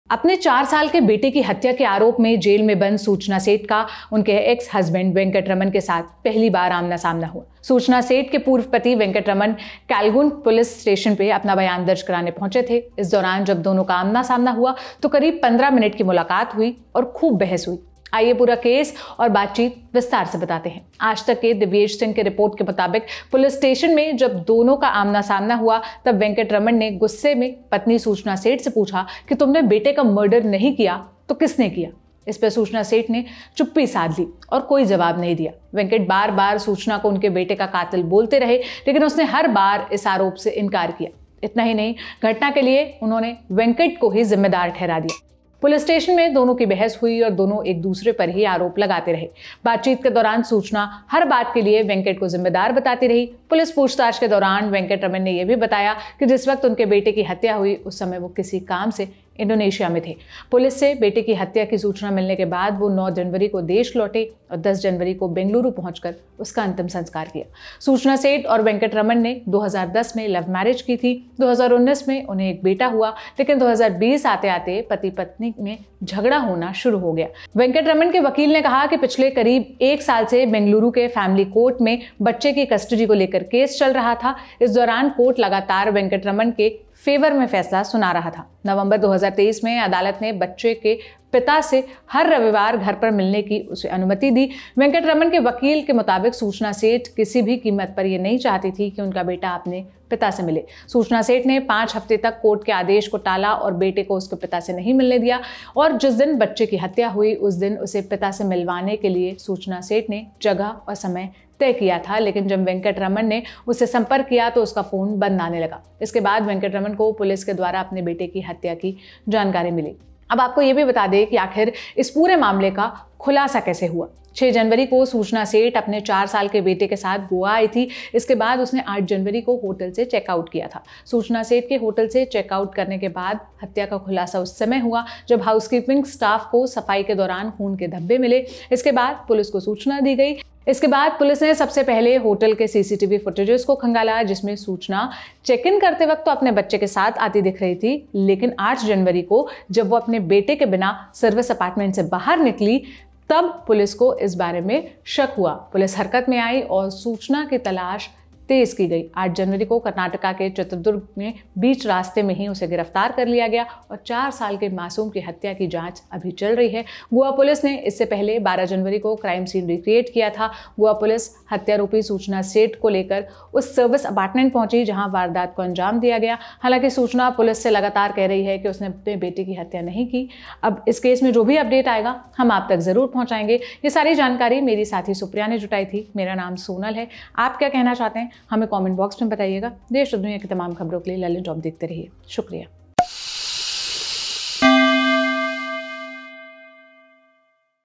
'बेटे को क्यों मार...' Goa Murder Case में Suchana Seth और उनके पति का Police के सामने आमना-सामना
For the first time, her husband Venkataraman met Soochna Seth, who was in jail for the murder of their 4-year-old son. Information Seth's ex-husband Venkataraman had come to Calangute police station to record his statement. During this time both of them came face to face. There was a 15-minute meeting between the two in which there was a lot of argument between Suchana Seth and her husband Venkat Raman.

Edited By: Prashant